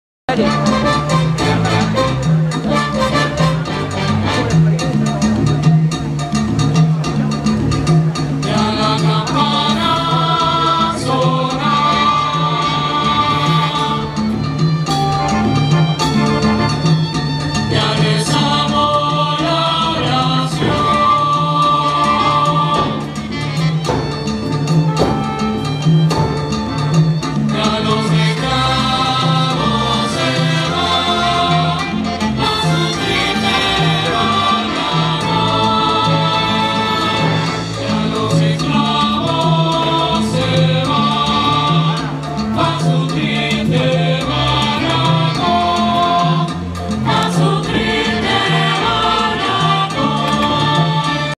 TROPICANA CUBA. The Best show Ever 2017
Salsa Holidays to Cuba with Leo Henriquez & Cubashe. Just a bit of the slavery Show 2nd part.